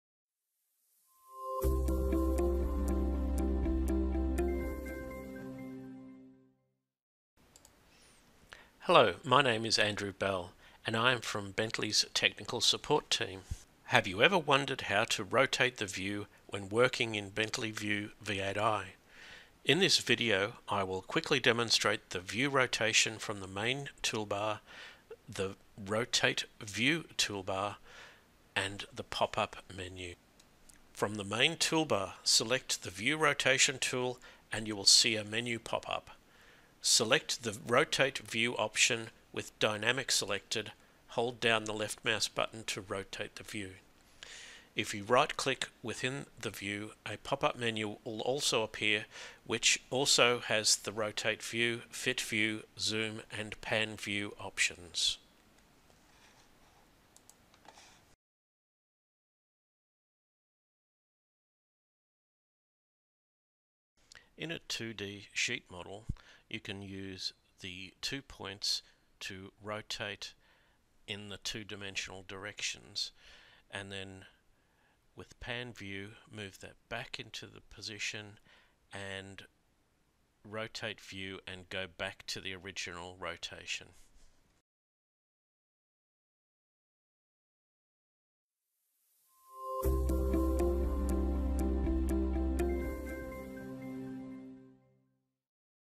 How to rotate a view in Bentley View V8i
Depending on whether you have a 2D or 3D design file open in Bentley View V8i different options for view rotation will display in the tool.

Become an accredited user, showcase your MicroStation skills as a valued asset in your organization.